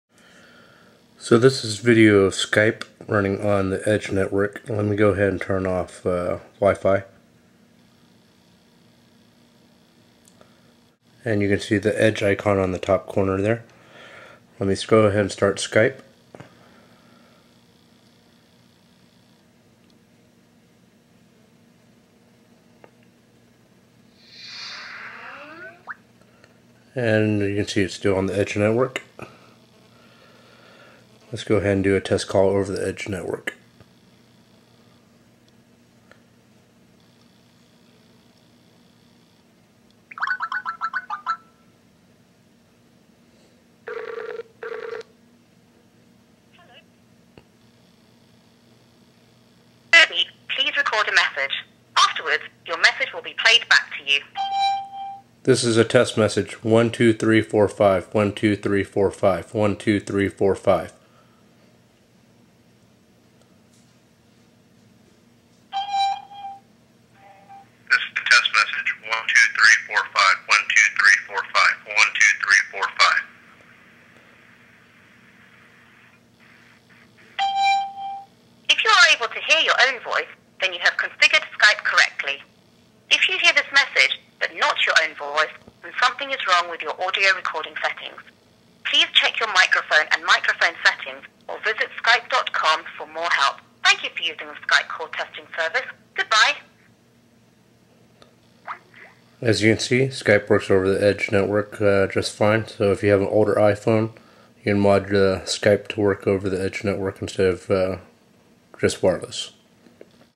Skype on the iPhone through EDGE (or 3G) Data Network
So Skype was released on the iPhone on March 31, 2009, but it is restricted to wifi use only.  However, it can be modified to run on the Edge and 3G data networks when wifi isn't available.  This is useful so you can Skype anywhere there is data service.  This video demonstrates running Skype on the Edge network on a first generation iPhone.  Unfortunately, if you have a GPRS data network it is too slow to handle VoIP.

To do this, you will need a jailbroken iPhone and the MobileSubstrate updated to the latest version so that VoIPover3G can be downloaded and used.  After VoIPover3G is installed, modify the VoIPover3G.plist to include com.skype.skype and then download the Skype application and you will be able to use it over your carrier's wireless EDGE or 3G data network.